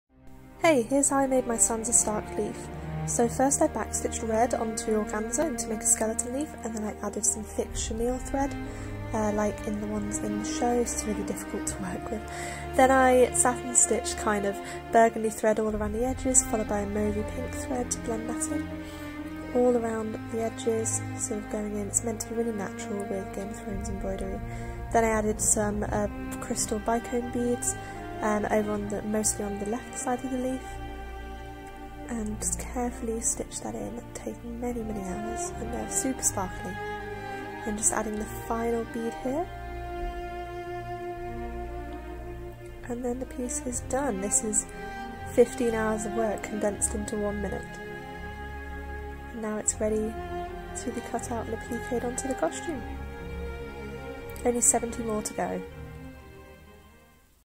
Sansa Stark Coronation Cosplay: Leaf Embroidery Tutorial-ish
15 hours of hand embroidery condensed into 57 seconds. if only I didn't still have like 50 leaves to make 😩

See the rest of my cosplay work at @amazoniancos on twitter and Instagram, and specifically embroidery work at @amazonembroidery on insta! 🥰